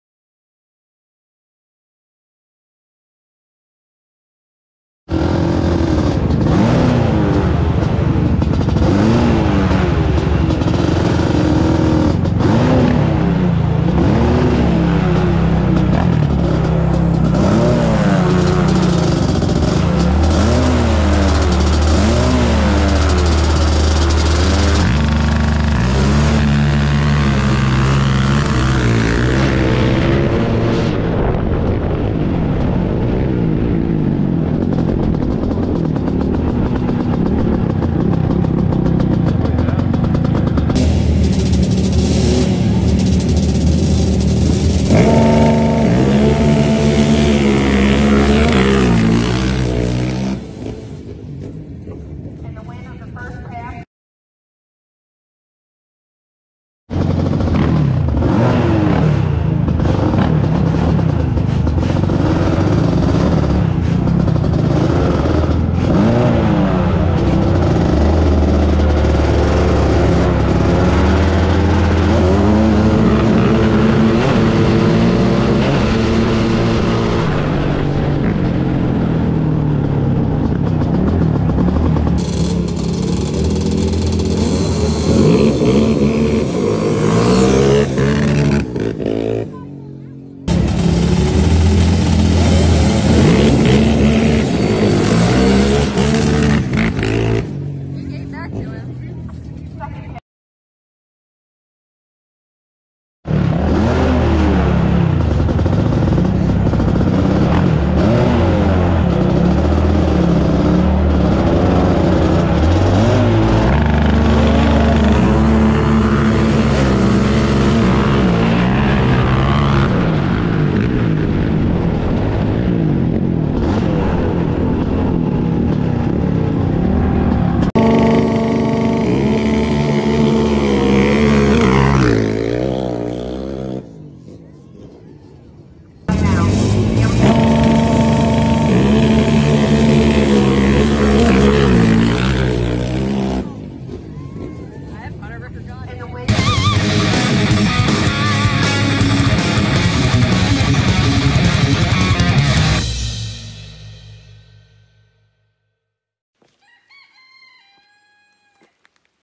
LT500R Quadzilla VS. Drag Built Raptor 700R
Going up against Hunter on Joshs Drag Built Raptor 700R on my Stock 1989 LT500R Quadzilla!

Mod’s:

LT500R: Full FMF Gold Series Exhaust.

Raptor 700R: SB/SS